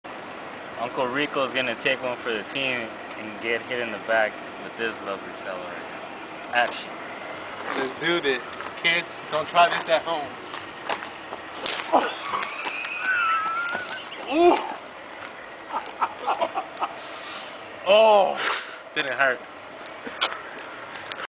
UncLE Rico Takes it in the back
george decides to hit me, rico, in the back with a damn piece of wood... didnt hurt though. that much... i think...